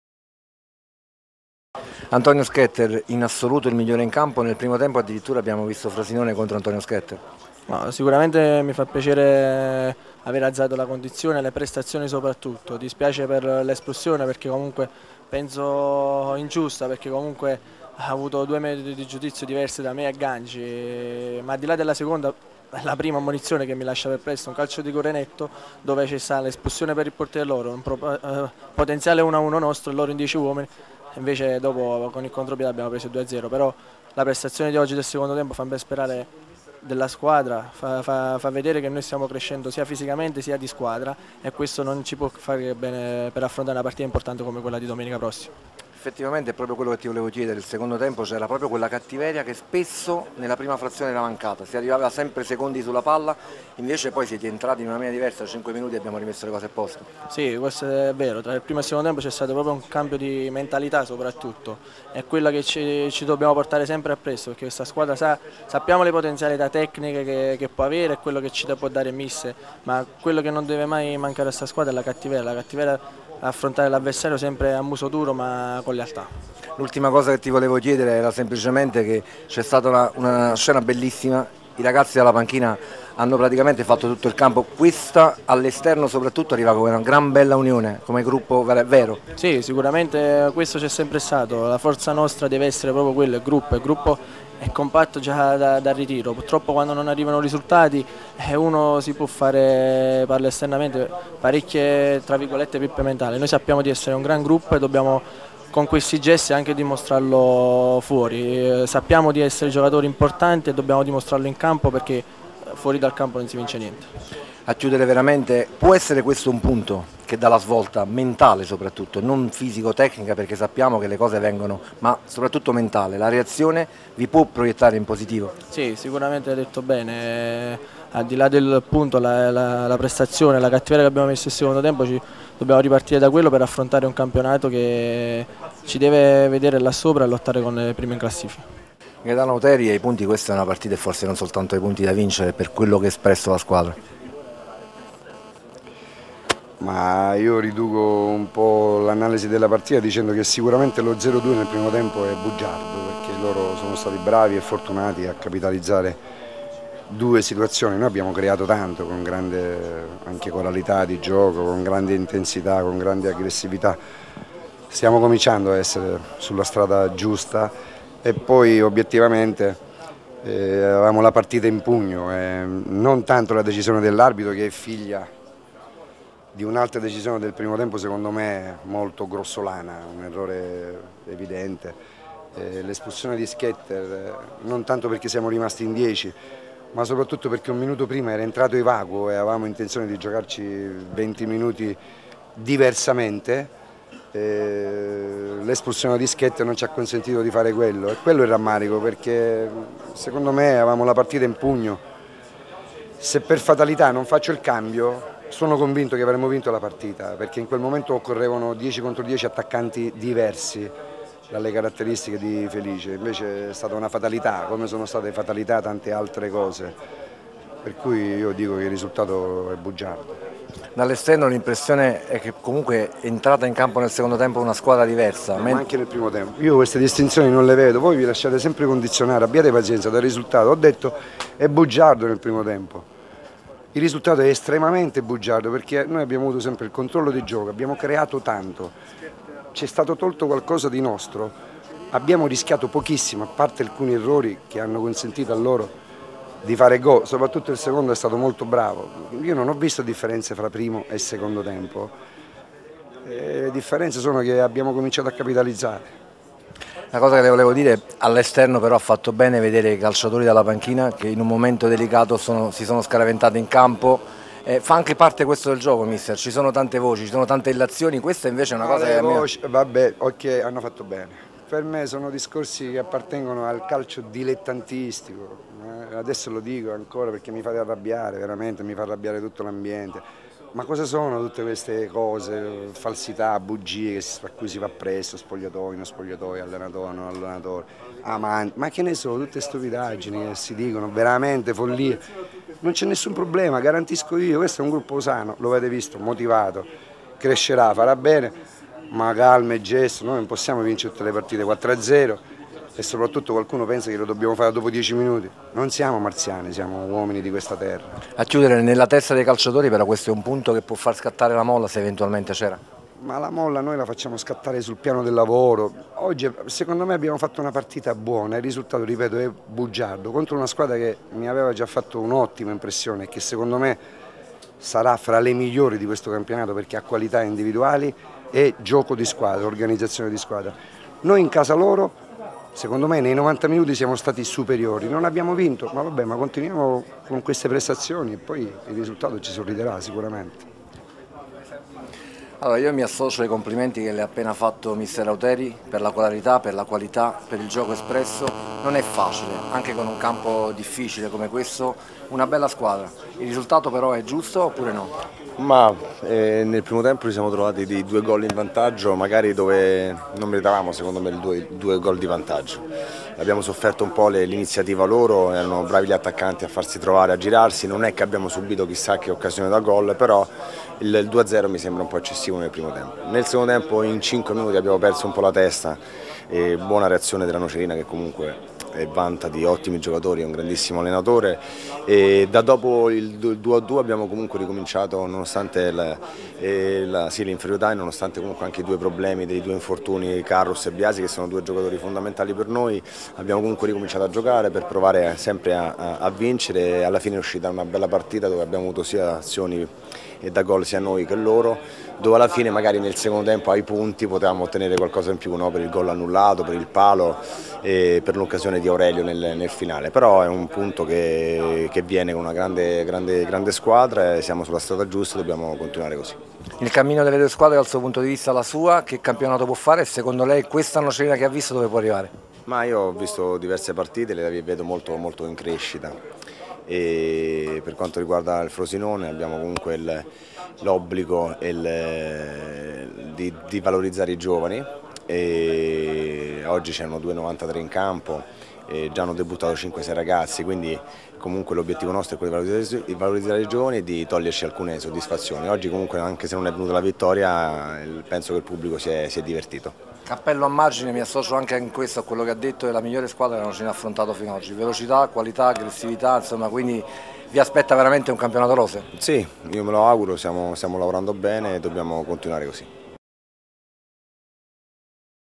interviste dopo gara Frosinone Nocerina
intervista a Antonio Schetter e a Mister Auteri.